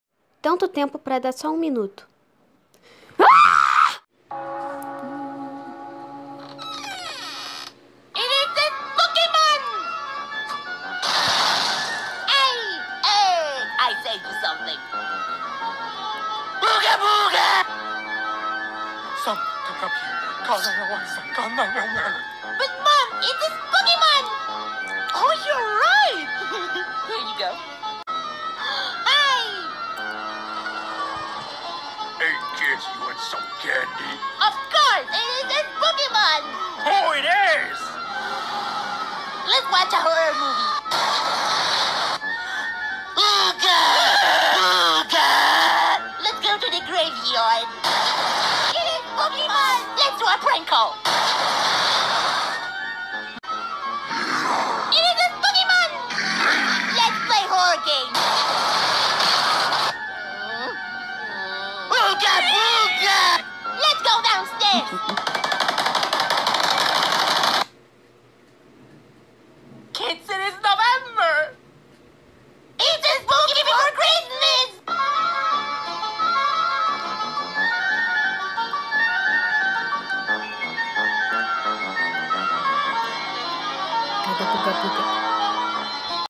🎃Feliz Mes Assustado 🎃(atrasado)
personagens que aparecem:

@mundo s de Davi 2 of hero

Biriba the star

David

Gi

Sonic. EXE

Sonic feio

Luigi bugado

no vídeo tem um link secreto vamos ver quem acha Mua HAHAHA